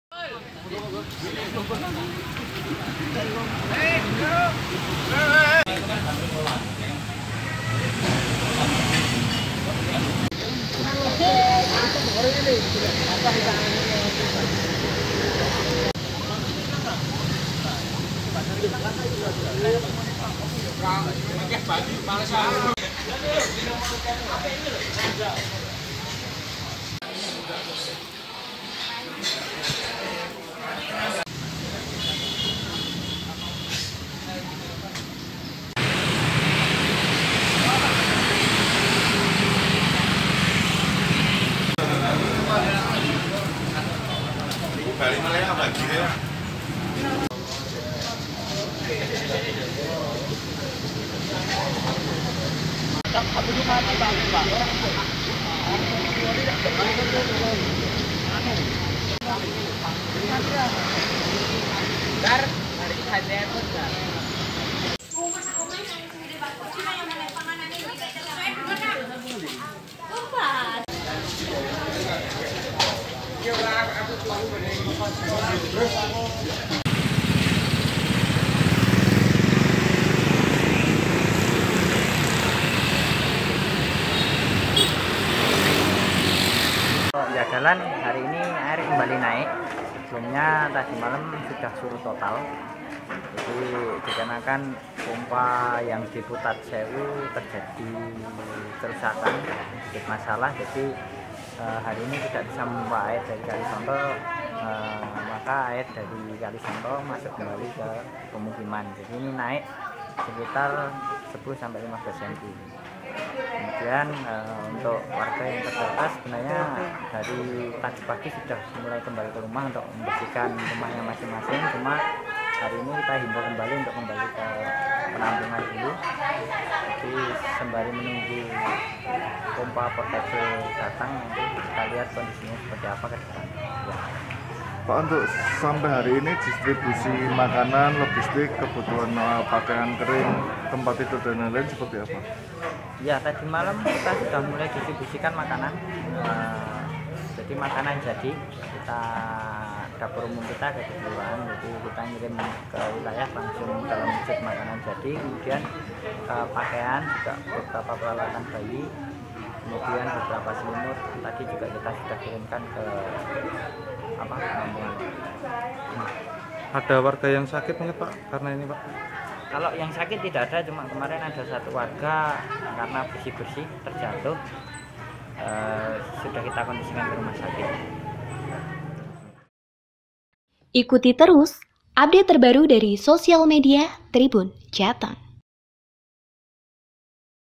Banjir Solo, Jagalan Sudah Surut Air Kembali Masuk Setinggi 15 Cm
Air di Kelurahan Jagalan, Kec Jebres, Kota Solo kembali naik.

Sebelumnya pada Kamis 16 Februari malam sudah surut total.

Air kembali naik dikarenakan pompa yang berada di Putatsewu terjadi kerusakan.

Pompa air tersebut terjadi kerusakan sehingga tidak bisa membuang air dari Kali Sonto di dekat pemukiman warga.

Dari data dari BPBD Kota Solo dari total warga yang terdampak sebanyak 4.440 jiwa pengungsi terpusat.